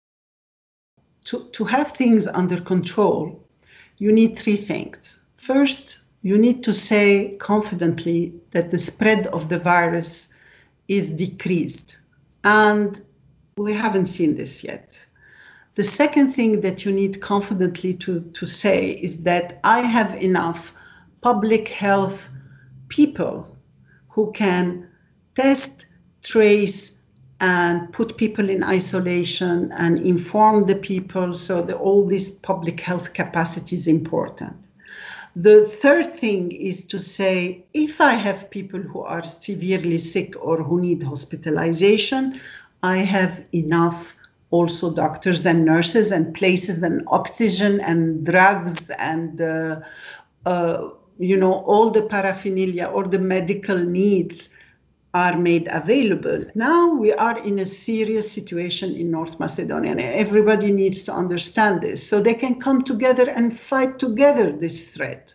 Кои три работи се потребни за да се стави вирусот под контрола?
Џихане Тавила, висока претставничка на СЗО во Скопје, објаснува за Гласот на Америка